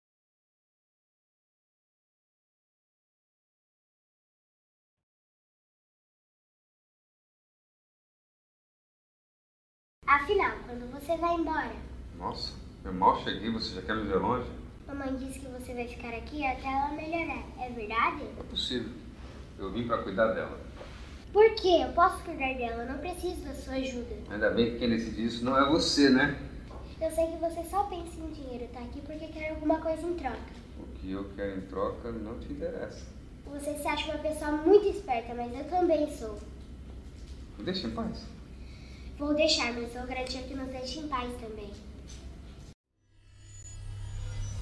Emanuele Hammarstrom
Workshop de Interpretação para TV com
a Produtora de Elenco Gabriela Rosales
Alunos em cenas com o ator Sidney Sampaio
Rio de Janeiro (Jan/2020)
Direção da Oficina de Atores Rio Sul: Alberto Oliveira
Filmagem: videobook.art.br